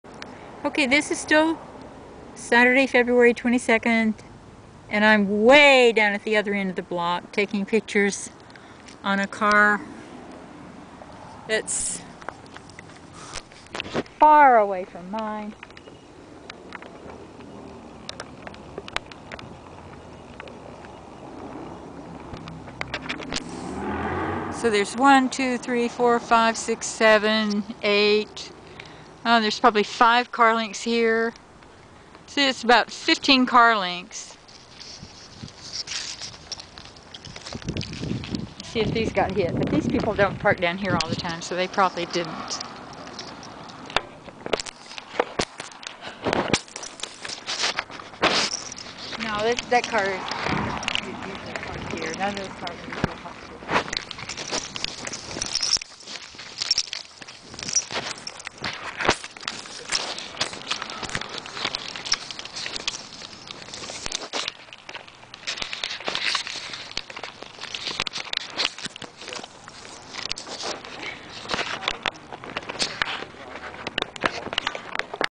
3 - Chemical Drops on Cars Sun 02-23-14 5:52PM Cols OH
Sunday, February 23, 2014 I photographed this 3rd of 3 videos of chemical drops on cars, sprayed from planes in Columbus, OH between OSU and  Clintonville on every car parked in the range of about 16 car lengths. Half were golden lumpy like cornbread batter that fell in rounder drops, like those I photographed on cars on January 14, 2013, but half were dark brown and thick about the consistency of chocolate brownie batter and fell in strips.